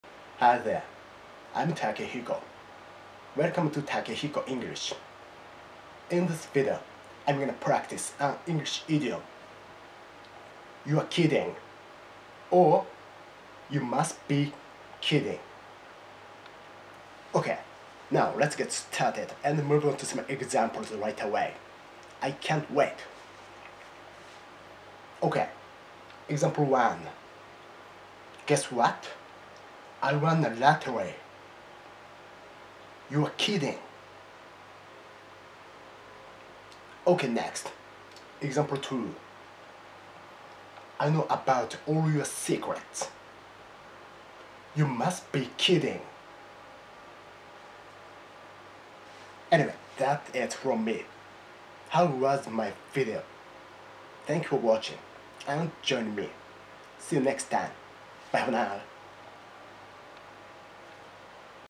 Takehiko English, "you're kidding/you must be kidding"
In this video, I'm practicing the English idiom "you're kidding/you must be kidding".